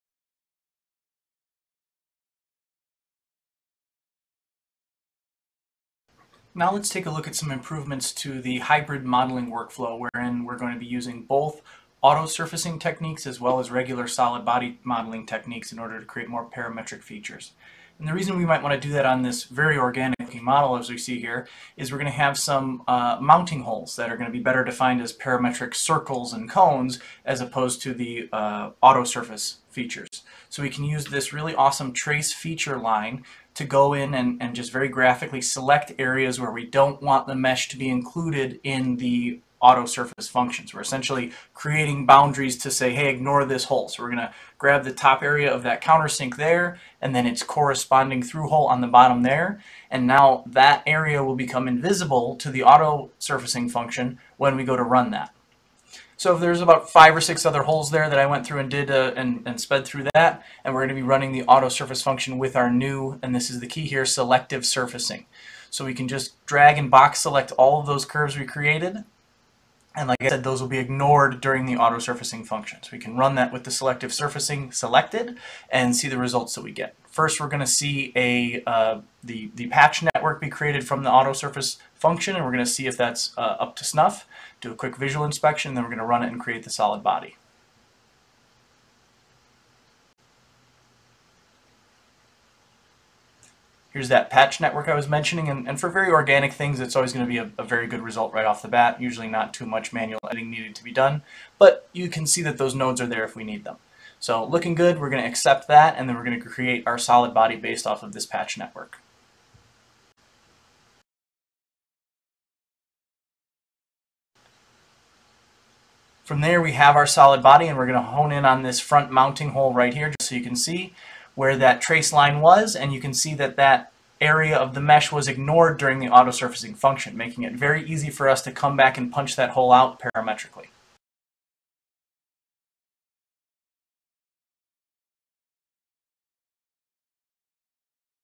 Hybrid Modeling Improvements – Geomagic Design X | What's New? 2020 - 2024.1
This video highlights some of the great enhancements that have been made to Geomagic Design X's hybrid modelling workflows over the past few years.

Speak to sales:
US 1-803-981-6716